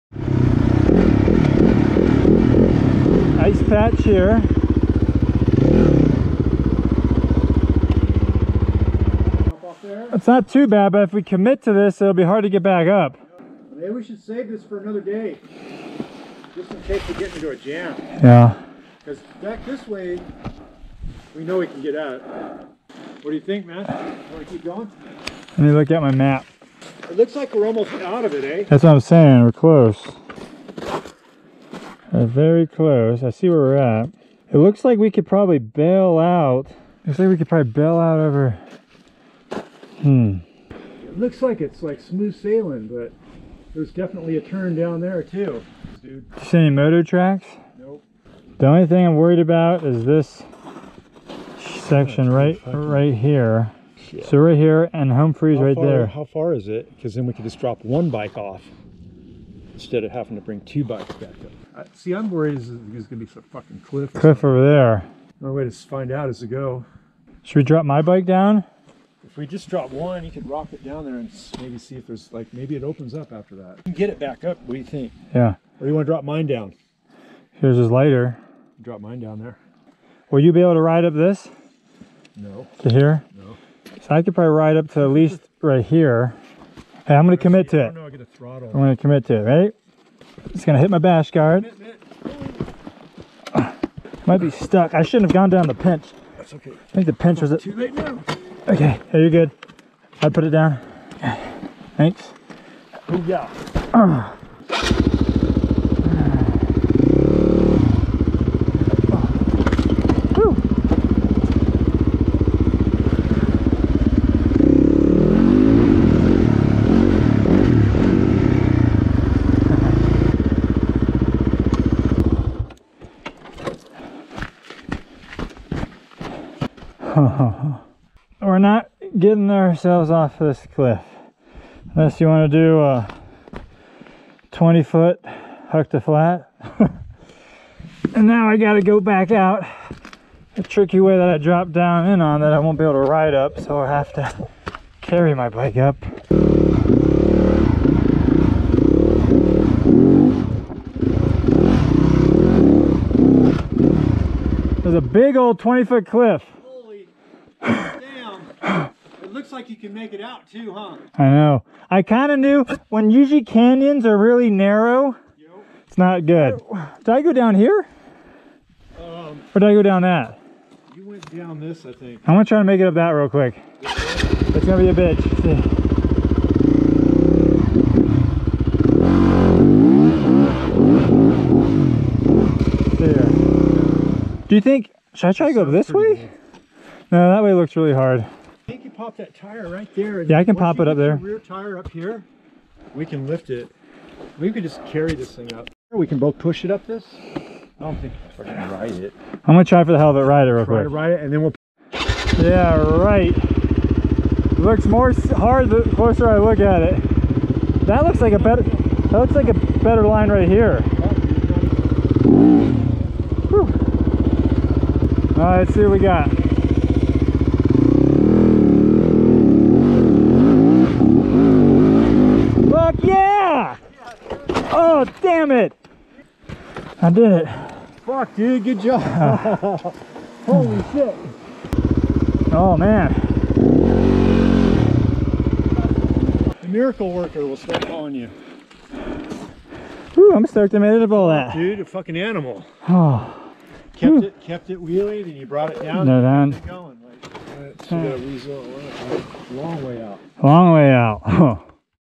YZ250FX | Decision Making
Ron and I are learning, we have blindly dropped into some stuff before just to get stuck and have an epic getting out.  We have dropped down scree fields just to get skunked and an hour later of dragging our bikes back up the slope to get out. This time we talked before we committed and only committed 1 bike and getting out wasn't too bad.  Funny to see our thought process unfold and shape.